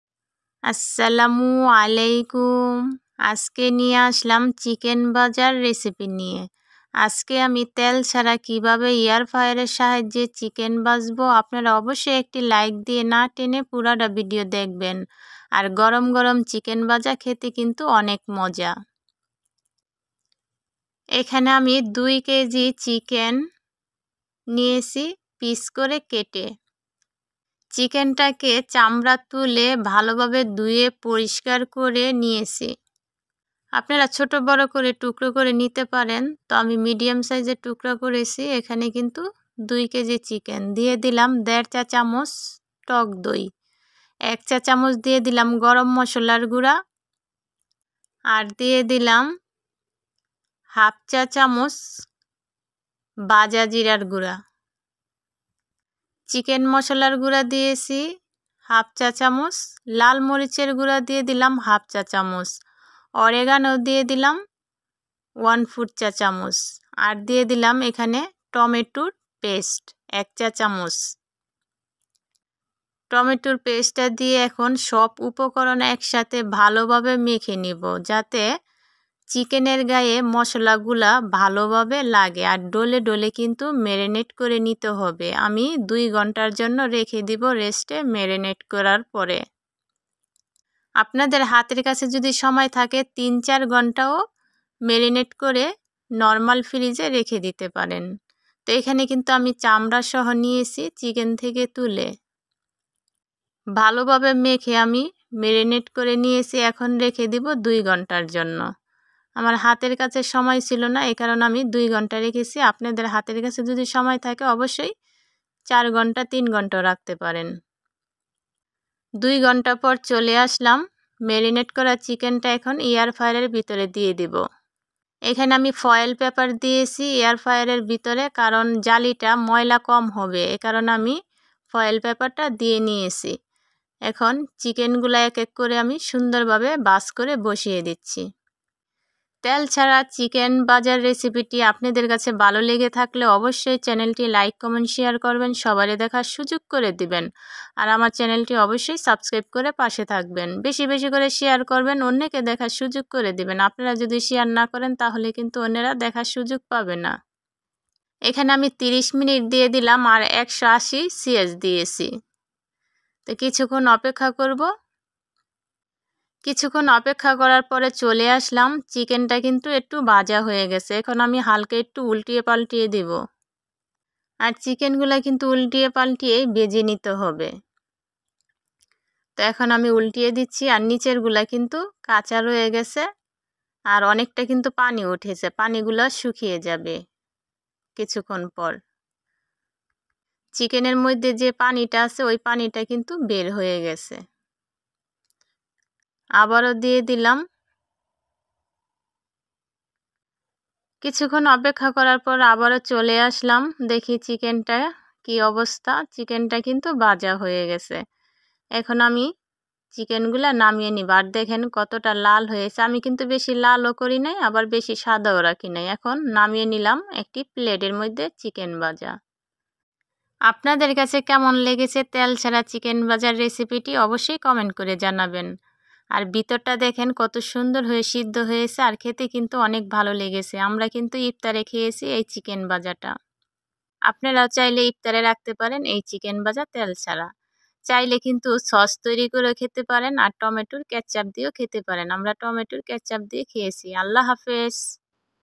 এয়ার ফ্রায়ার চিকেন ভাজা তেল ছাড়া।Easy Healthy Fried Chicken। how to cook air fryer chicken vhaja ।
এয়ার ফ্রায়ার চিকেন ভাজা তেল ছাড়া।Easy Healthy Fried Chicken। how to cook air fryer chicken fry।

 ২ কেজি চিকেন ভালোভাবে ধুয়ে পরিষ্কার করে নিয়েছি প্রথমে।
চিকেন টাকে মেরিনেট করে নিয়েছি, স্বাদ মত লবণ, দেড় চা চামচ টক দই, এক চা চামচ গরম মসলার গুড়া, হাফ চা চামচ বাজা জিরার গুড়া, হাফ চা চামচ চিকেন মশলার গুড়া,এক চা চামচ লাল মরিচের গুঁড়া, এক চা চামচ টমেটো পেস্ট, ২ ঘন্টা রেস্ট রেখেছিলাম মেরিনেট করে।